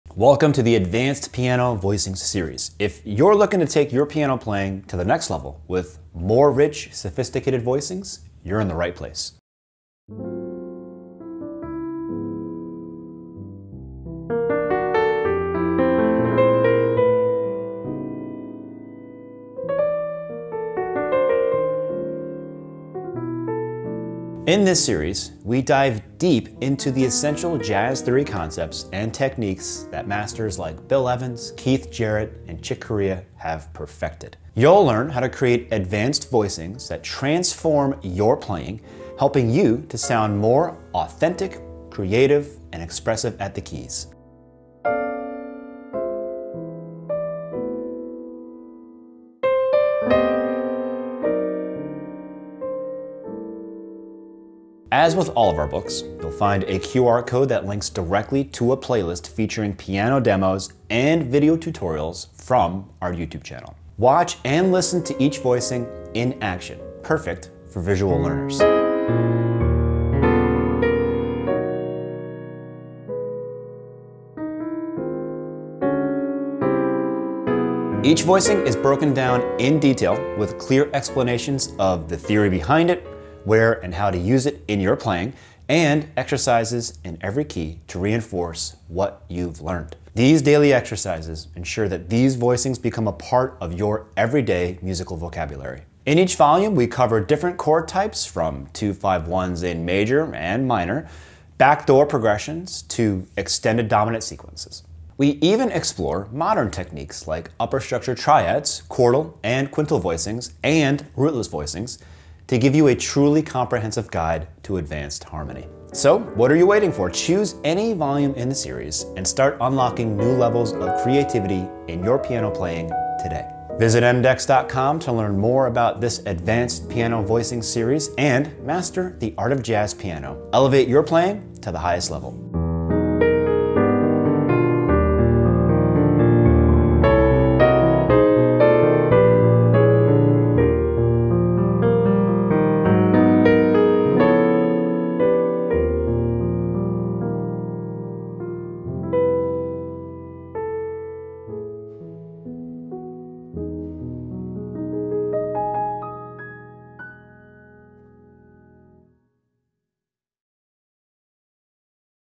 Advanced Jazz Piano Voicings Vol.1, 2 & 3 | Take A Look Inside
Advanced Piano Voicings Vol.1, 2 & 3

This is a comprehensive practice method with exercises in all keys dedicated to advanced jazz piano voicings. By regularly practicing this workbook, you will learn advanced jazz piano voicings in all keys within the context of various cadences—such as 2-5’s and 2-5-1’s—and expand your jazz vocabulary by incorporating effective piano voicings in the genre.

While in vol.1, we studied a set of 12 advanced piano voicings; in vol.2, we will be practicing cadences with 2 or 3 chords; in vol.3 we take a deep dive on 2-5-1's in minor, backdoor progression reharms & upper structure triad pairs from the altered scale and the symmetric diminished. We’ve included exercises for each cadence in all keys, an explanation of how the voicings are built, and guidance on how to think about them effectively in order to incorporate them into your playing.

A short melody harmonized, with all the voicings notated, is also included in several keys. Additionally, you'll find a lead sheet with the melody and chord symbols in all keys to practice playing the voicings by heart—an essential step if you want to truly incorporate these voicings into your playing.